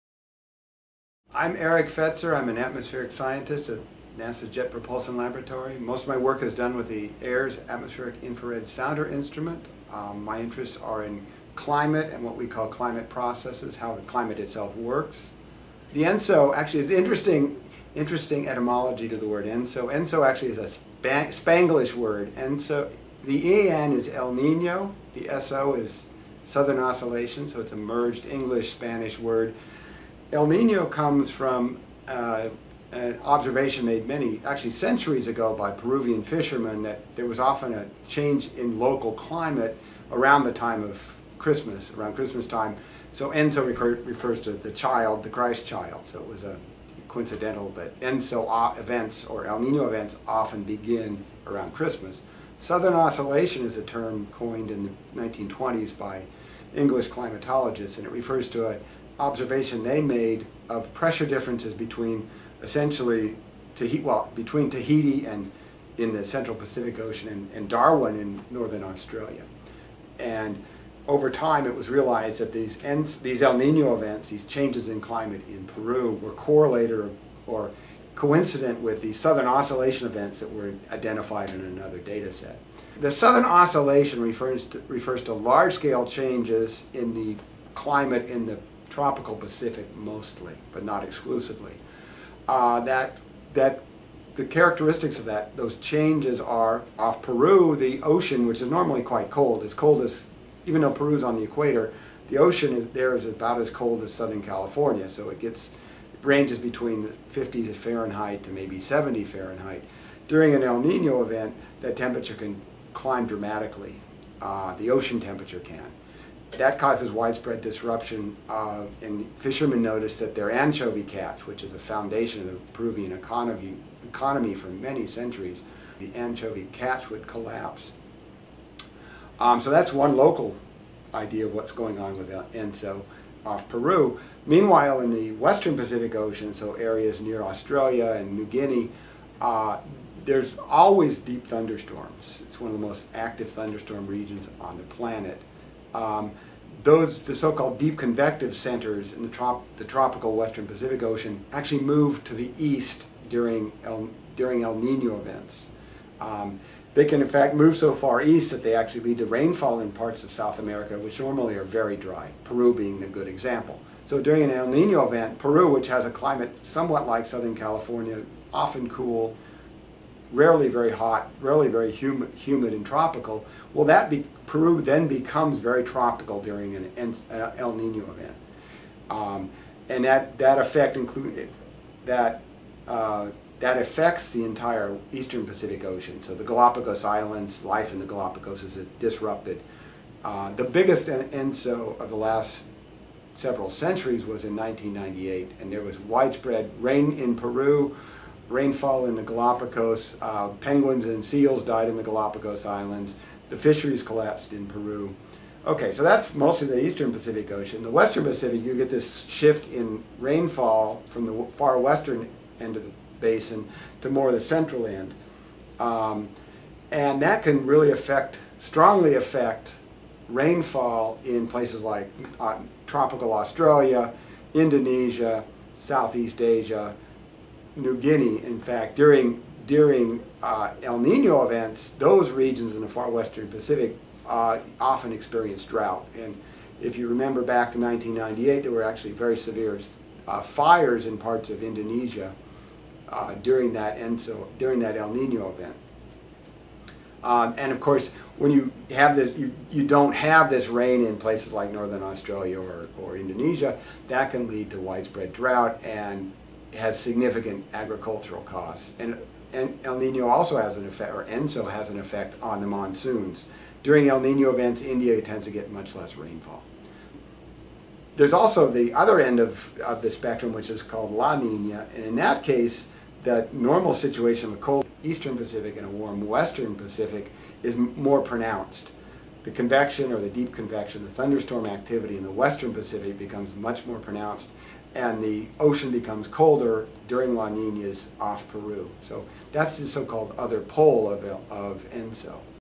What-is-ENSO.mov
Dr. Eric Fetzer, Project Scientist of the Atmospheric Infrared Sounder (AIRS) Project at NASA's Jet Propulsion Laboratory, gives an overview of the El Nino Southern Oscillation.